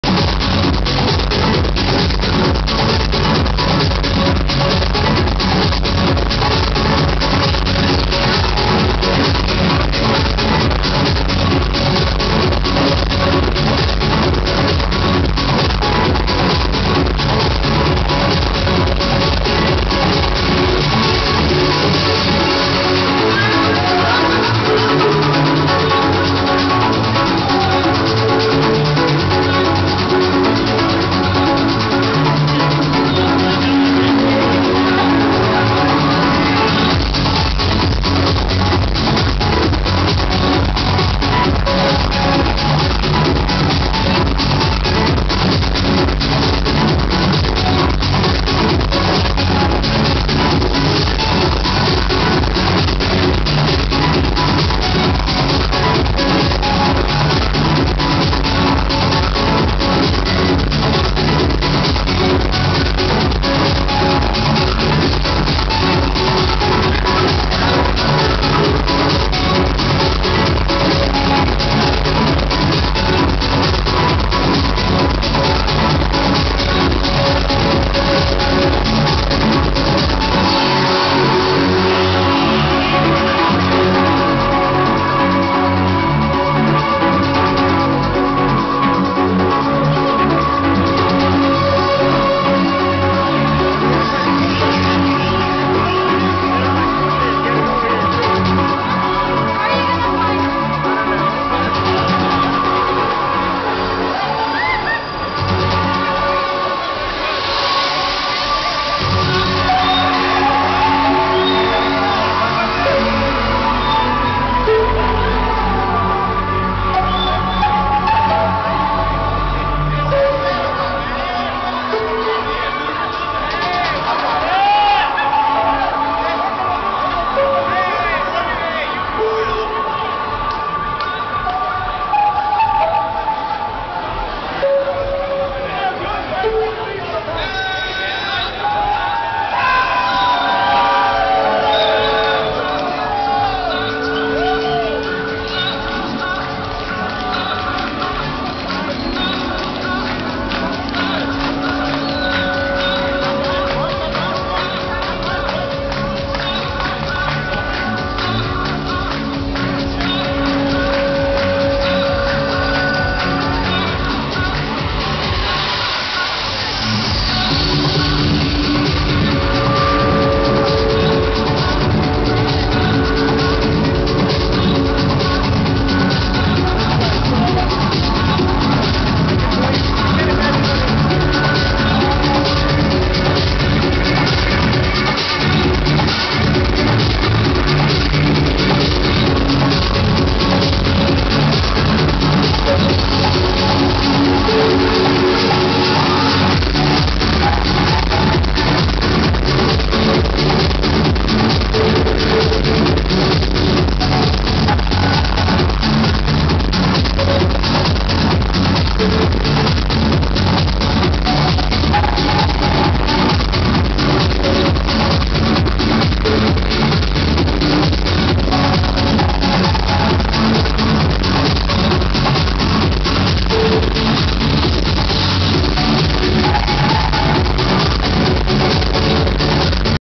ATB Live in San Francisco at Club 1015 Folsom Oct 7 2006 (Preparty)
A dj that spun before ATB Live in San Francisco at Club 1015 Folsom Oct 7 2006. Sorry for the quality, the camera can't record such loud sound well.

Please help identify the tune he's playing, it's AMAZING!